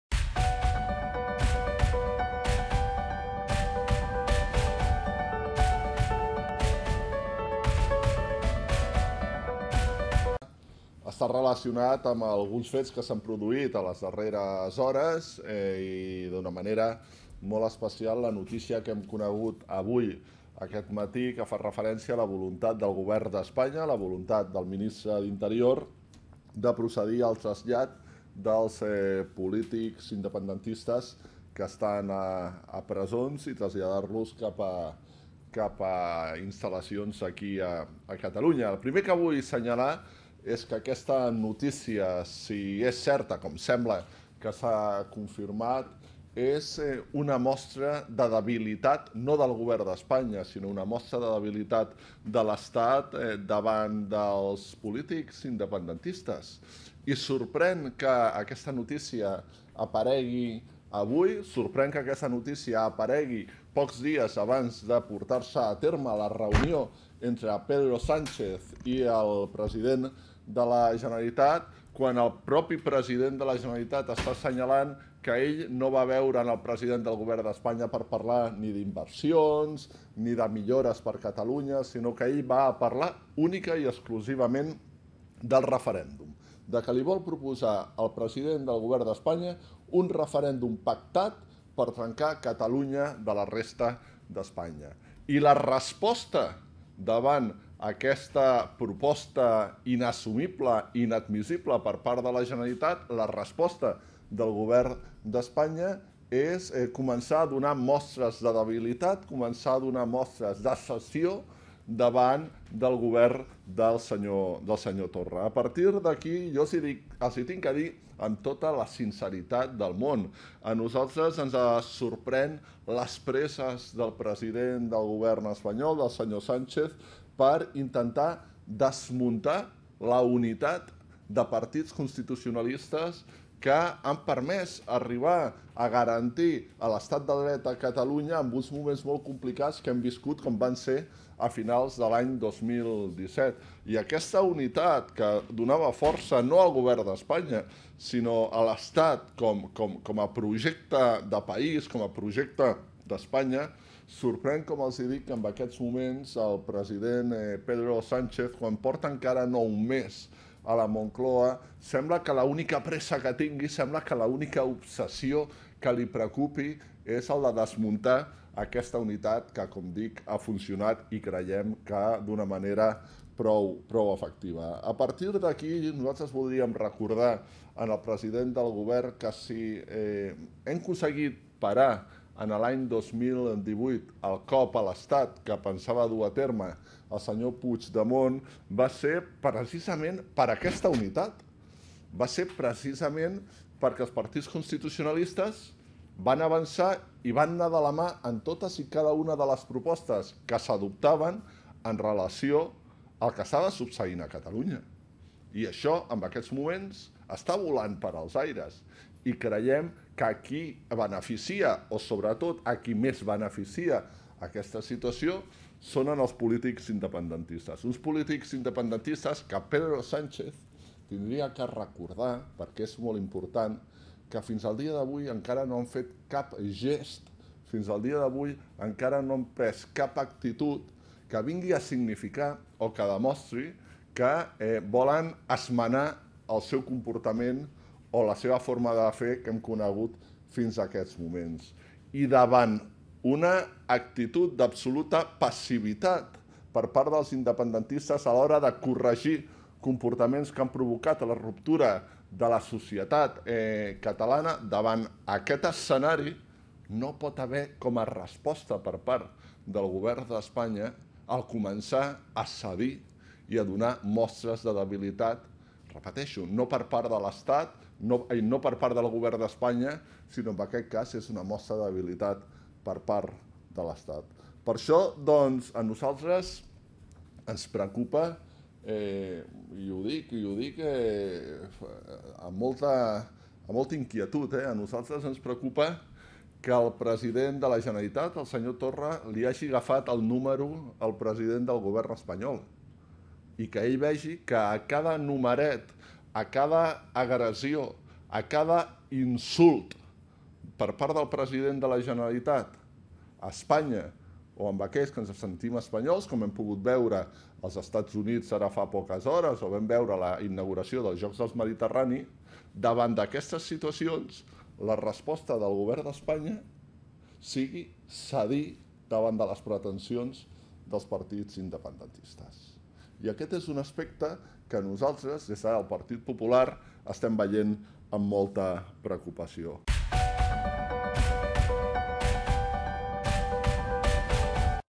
Garcia Albiol El Acercamiento de los Presos es una muestra de debilidad
Rueda de prensa de Garcia Albiol del PP en el Parlament 28 de Junio de 2018:  El Acercamiento de los Presos es una muestra de debilidad del Estado delante de los políticos independentistas. Fuente Canal Parlament.